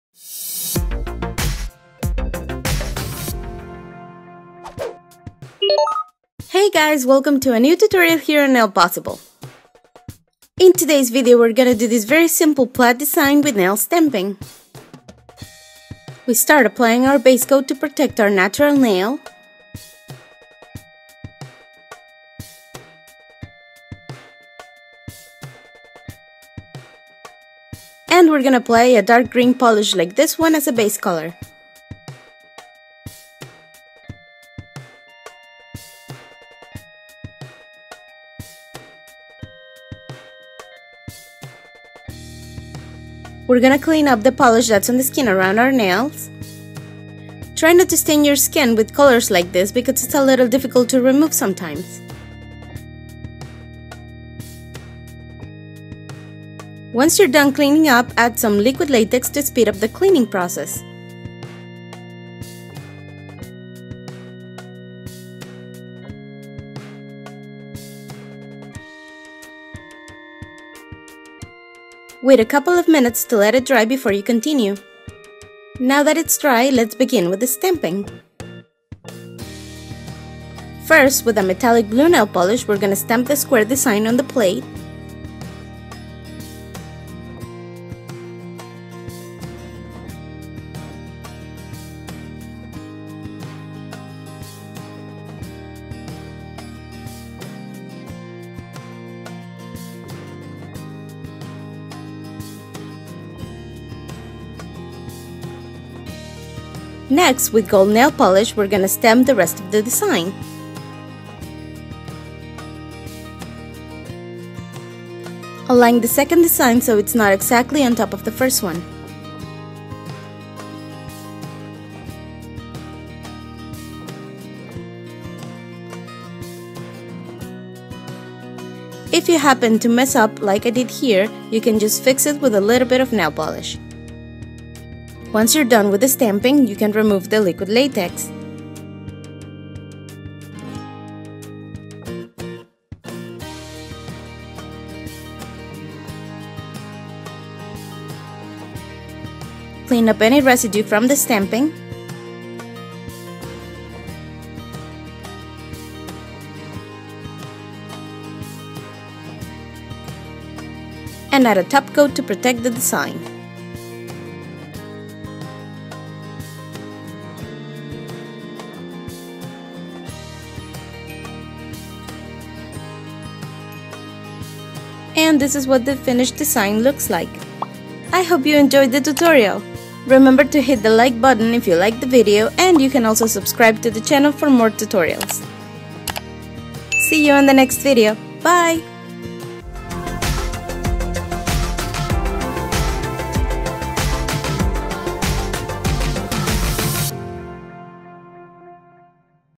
Dark Green Plaid Nail Art Design
Hello There! In this tutorial we're going to do this very simple plaid design using nail stamping, it's super simple but I love how it looks! I hope you enjoy it!

Products I used in this video:

★Base coat: Revlon ColorStay Gel-Smooth Base Coat
★Dark green nail polish: Nicole by O·P·I Khloe Had A Little Lam-Lam
★Metallic blue nail polish (stamping): Maybelline Color Show Navy Narcissist
★Gold nail polish (stamping): Sinful Colors Gold Medal
★Top coat: Seche Vite Dry Fast Top Coat
★Tweezers